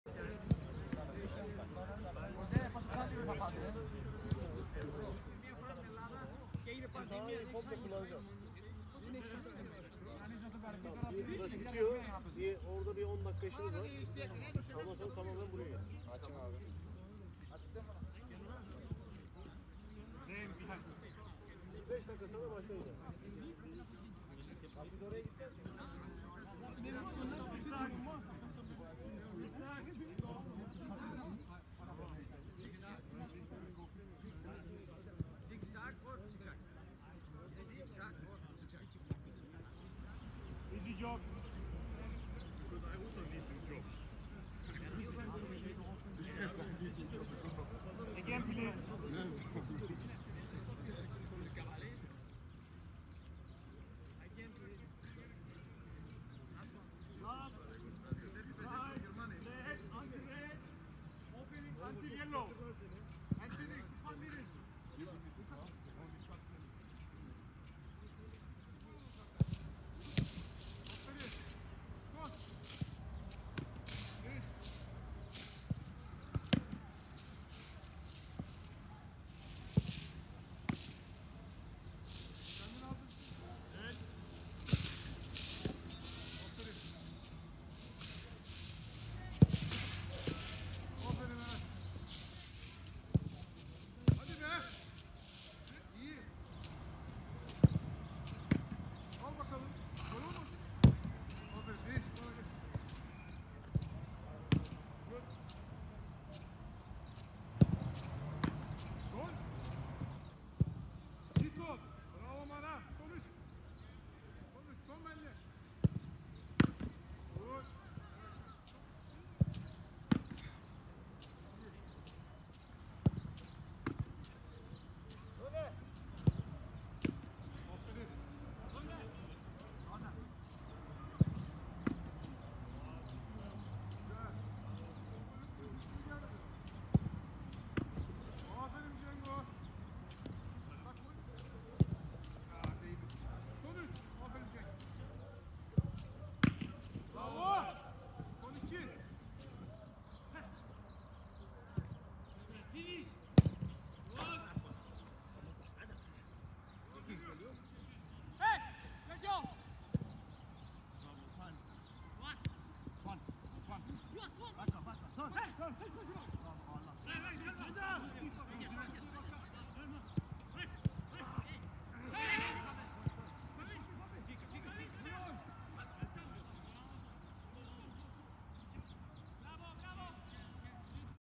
Alanyaspor hazırlıklarını grup halinde yaptığı idmanla sürdürdü
Süper Lig ekiplerinden Aytemiz Alanyaspor, çalışmalarına bugün grup halinde yaptığı antrenmanla devam etti.
Süper Lig’in kalan 8 haftası için hazırlıklarına Teknik Direktör Erol Bulut’un gözetiminde Alanyaspor Cengiz Aydoğan Tesisleri’nde devam eden Aytemiz Alanyaspor, bugün ilk kez tek grup olarak çalıştı. İdmana ısınma hareketleriyle başlayan turuncu-yeşilli ekip, daha sonra tam sahada taktik çalışmasıyla devam etti. Hafif ağrıları bulunan Salih Uçar takımdan ayrı düz koşu yaparken, Emircan Altıntaş ise salonda çalıştı. Alanyaspor, hazırlıklarına yarın yapacağı antrenmanla devam edecek.